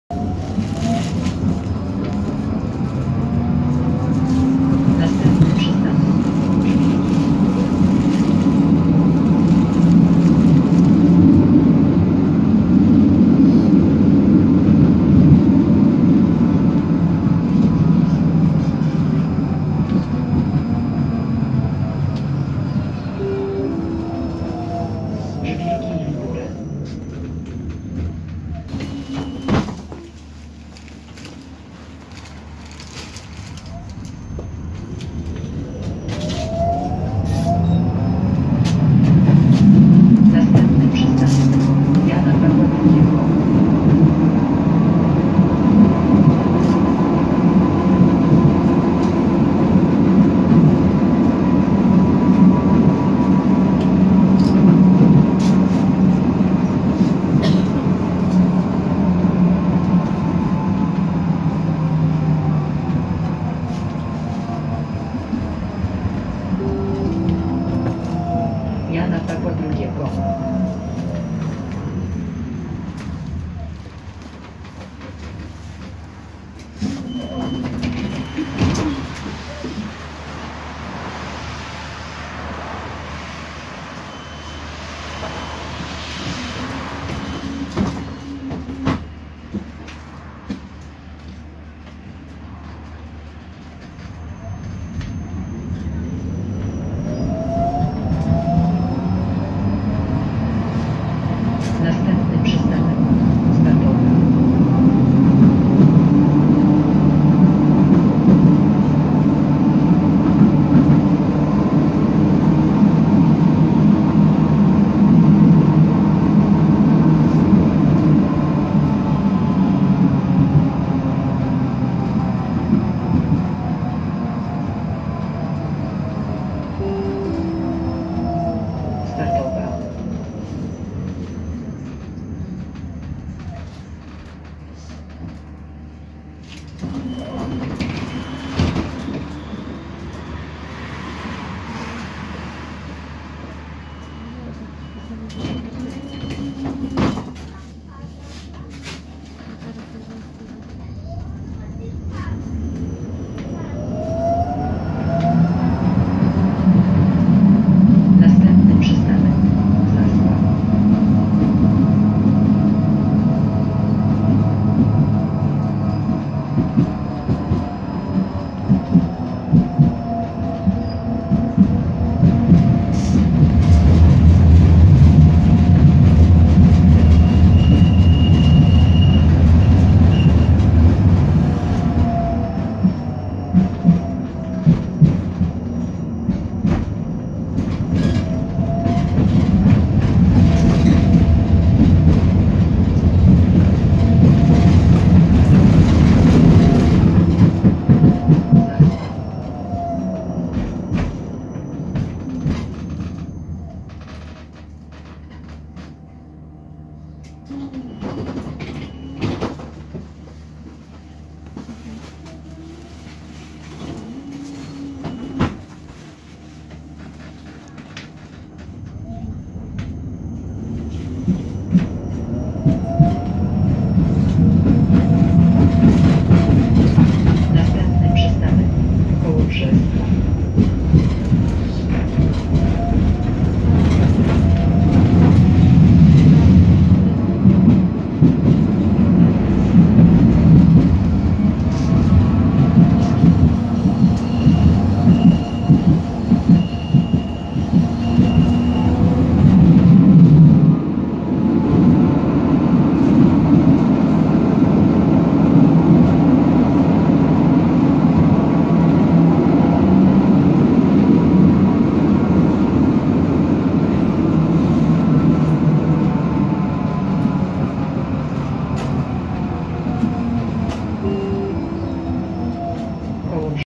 [GAiT] Düwag N8C-AC2 #1171 - linia 4, kierunek Strzyża PKM
-- TRASA --
0. Bajana
1. Żwirki i Wigury
2. Jana Pawła II
3. Startowa
4. Zaspa
5. Kołobrzeska

Gdańska Komunikacja Miejska 2020